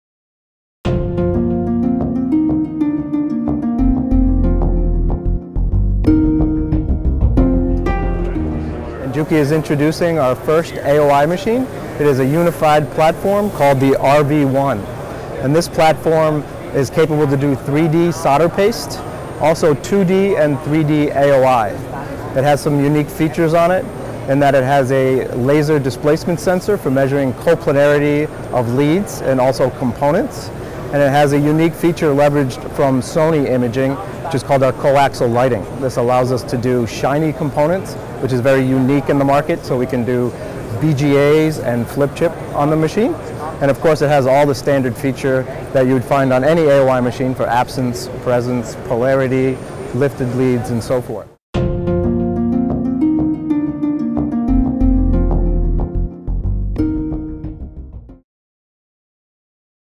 RV-1 | PWB Visual Inspection Machine (AOI/SPI) [Part 2]
Equipped with a "Clear Vision Capturing System", the RV-1 is capable high-speed and high-precision inspection.

The RV-1 captures super clear images using a large sized ring light and coaxial light and enables easy operation by using a newly developed "Template Mode".